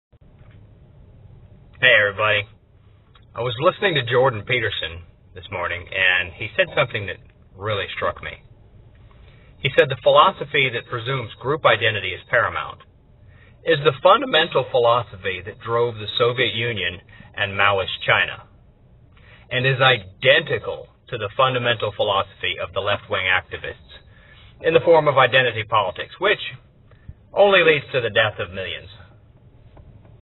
Leftist Identity Politics is Identical to What Caused the USSR and Mao's China
The philosophy  that presumes group identity is paramount is  the fundamental philosophy that drove  the Soviet Union and Maoist China and is identical to the fundamental philosophy of the left wing activists (identity politics) which.. only leads to the death of millions.